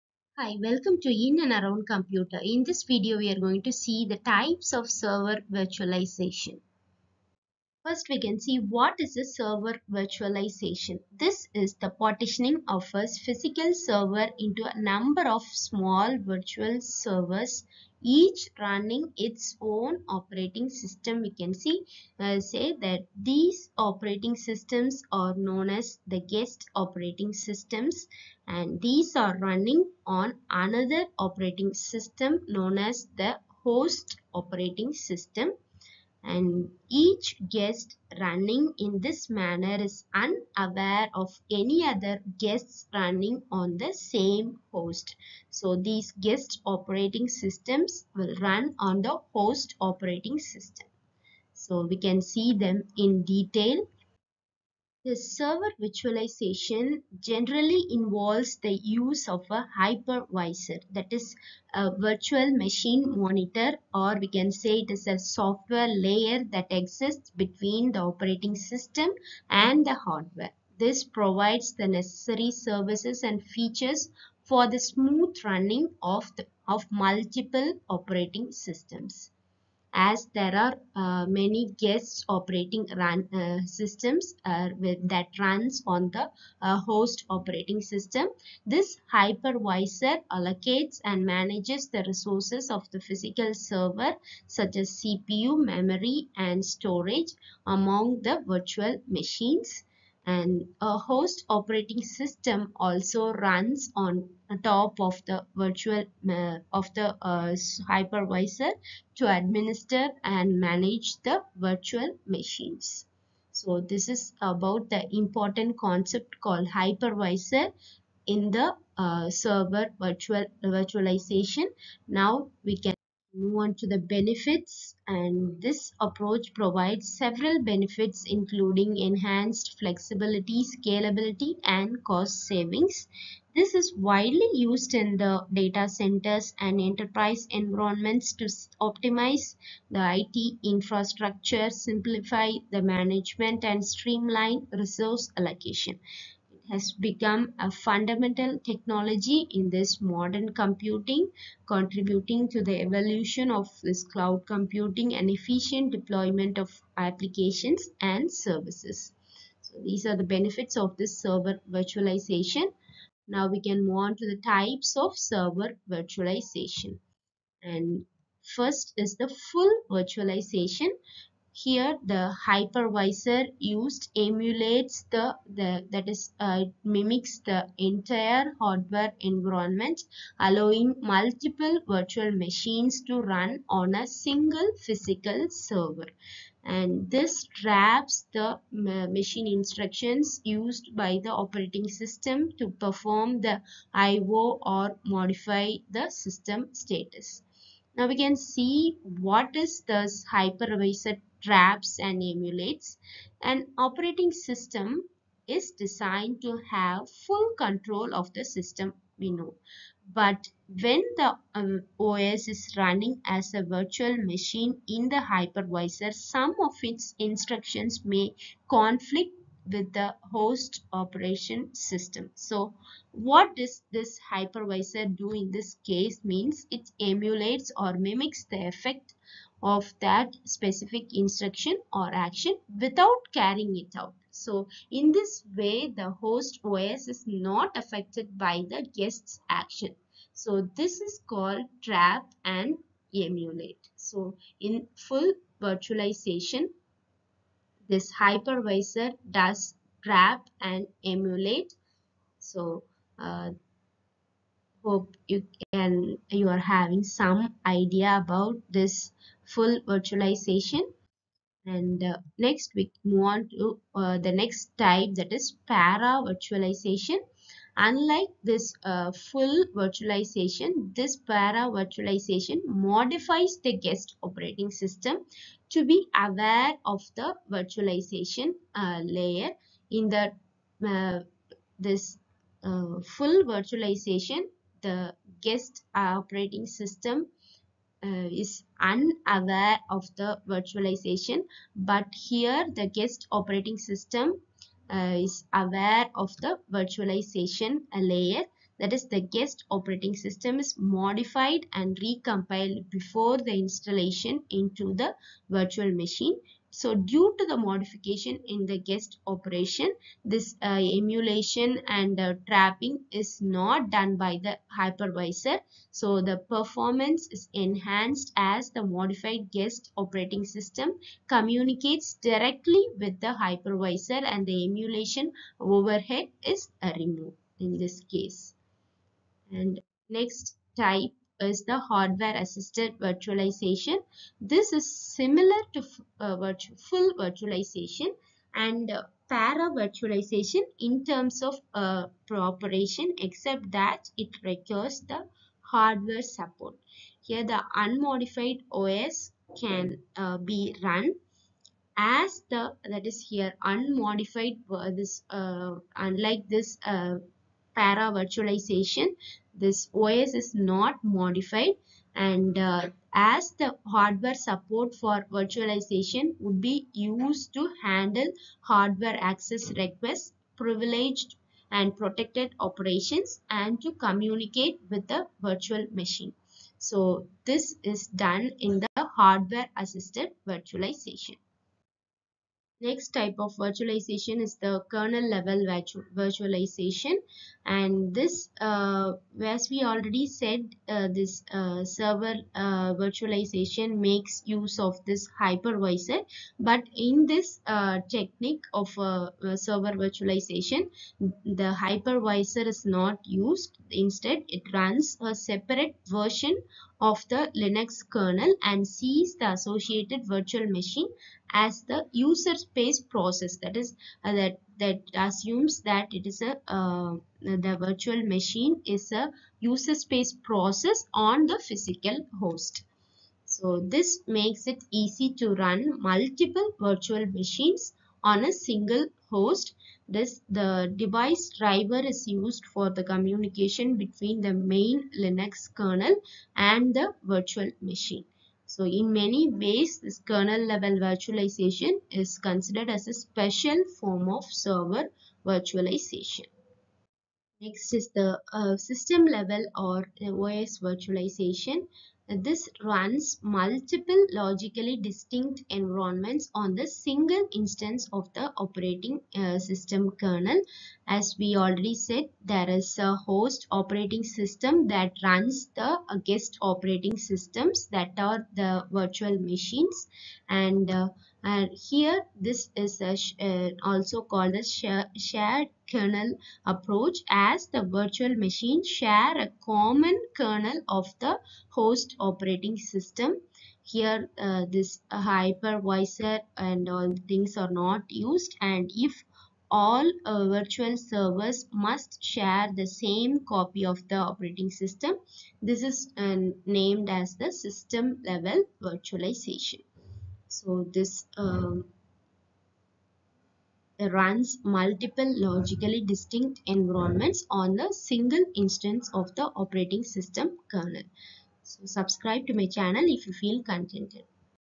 What is Server Virtualization? Types of Server Virtualization Benefits of Server Virtualization
This process involves the use of a hypervisor, which is a VMM(virtual machine monitor) a software layer that exists between the operating system and hardware. It provides the necessary services and features for the smooth running of multiple operating systems.
 The hypervisor allocates and manages the resources of the physical server, such as CPU, memory, and storage, among the virtual machines.
 A host operating system also runs on top of the hypervisor to administer and manage the virtual machines.